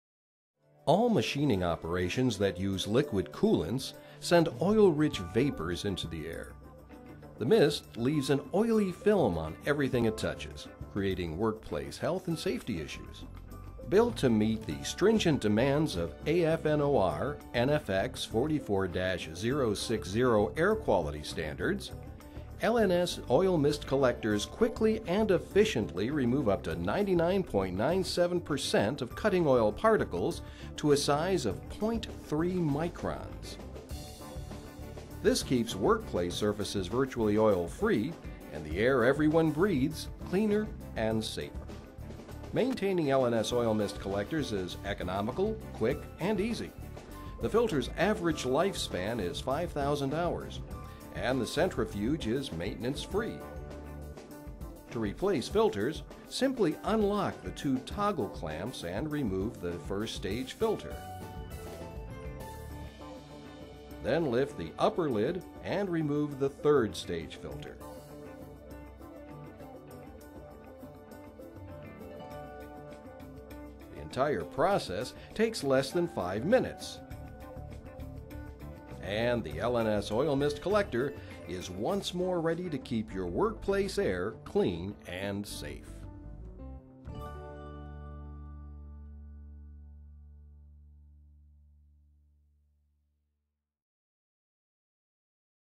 Oil Mist Collector
The oil mist collector Fox WS series is the perfect solution for the elimination of oil mist typical of metal cutting applications. With its small and compact dimensions the WS series integrates perfectly with the machine tool design and thanks its control panel it can be easily interfaced.